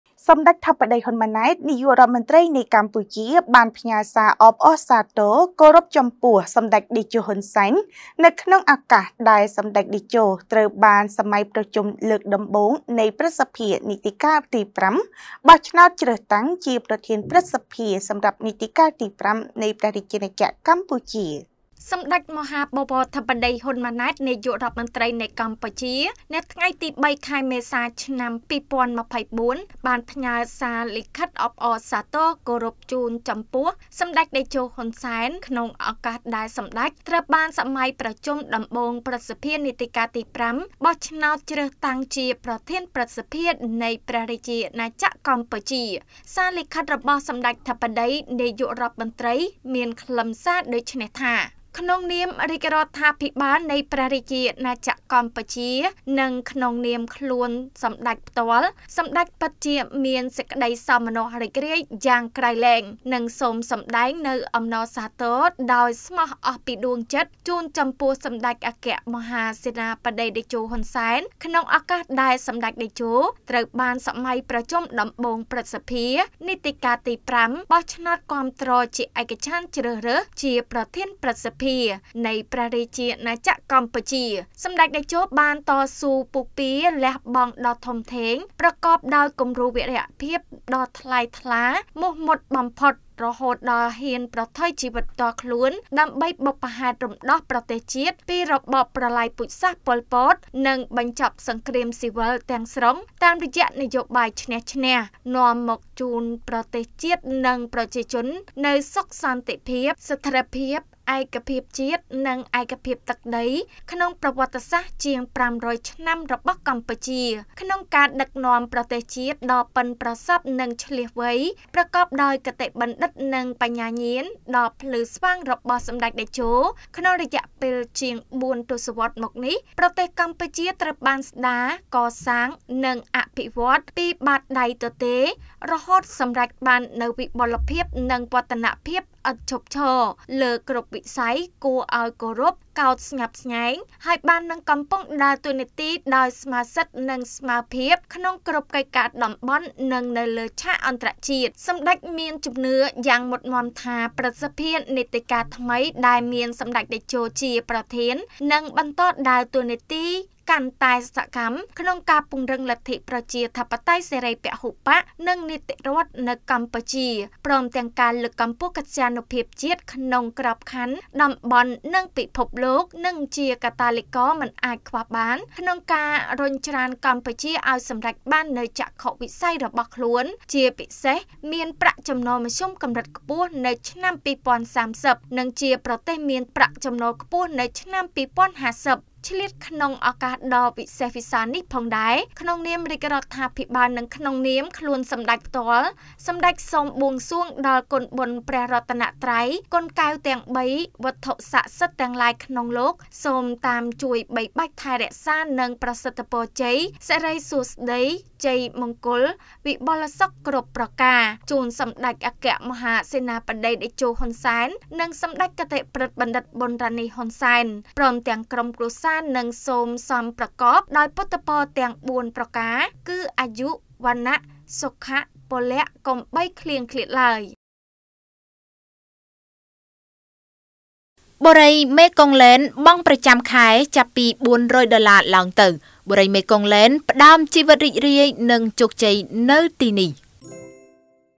សម្ដេចធិបតី ហ៊ុន ម៉ាណែត ផ្ញើសារលិខិតអបអរសាទរគោរព ជូនចំពោះសម្តេចតេជោ ហ៊ុន សែន.
សម្ដេចធិបតី ហ៊ុន ម៉ាណែត ផ្ញើសារលិខិតអបអរសាទរគោរព ជូនចំពោះសម្តេចតេជោ ហ៊ុន សែន ក្នុងឱកាសត្រូវបានសម័យប្រជុំដំបូង ព្រឹទ្ធសភា បោះឆ្នោតជ្រើសតាំងជា ប្រធានព្រឹទ្ធសភា នីតិកាលទី៥
* រាយការណ៍ដោយ កញ្ញា ថាន រស្មី និងកញ្ញា កេត សត្យា